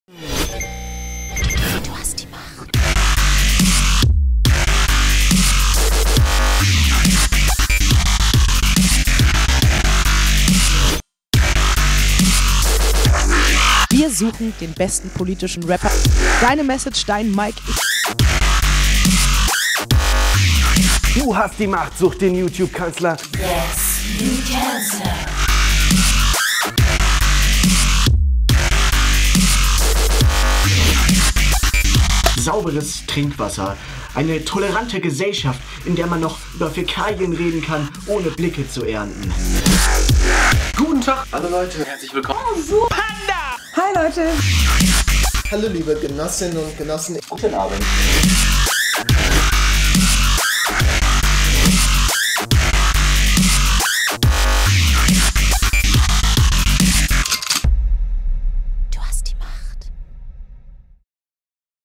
Willkommen auf DU HAST DIE MACHT!
Cannabis legalisieren? Freies Internet oder Kontrolle? Mehr Rechte für Asylsuchende?

Wir gucken auch gerne Katzenvideos, aber hier geht's dann vielleicht eher um Tierversuche als um Dubstep-Kätzchen. Wir sind Deine Plattform für politische und gesellschaftliche Themen.

Hier kannst du zum jungen Producer oder Streetblogger werden! Oder die Redaktion mit Themenvorschlägen und Kritik bombardieren. Hier kannst du einem Politiker mal richtig die Meinung sagen, Wettbewerbe z.B RAPutation oder YouTube-Kanzler gewinnen und Deine Stimme nutzen.

Die DU HAST DIE MACHT Formel

(DU x Redaktion x K-Team) + Politiker : YouTube = DHDM

Das K-Team, das ist die Gruppe um den ersten und einzigen YouTube-Kanzler (Dailyknoedel) -- Das K-TEAM = 10 YouTuber - 10 Formate! Unsere feste Redaktion besteht aus zwei Redakteurinnen Anna & Franzi und unseren Jungs (Norman & Ole), die kümmern sich um Kamera und Schnitt.

Web: duhastdiemacht.de

DU HAST DIE MACHT steht für Respekt. Beleidigende oder diskriminierende Kommentare werden gelöscht. Wir behalten uns vor, Kommentare zu entfernen, die mit dem Video nichts zu tun haben.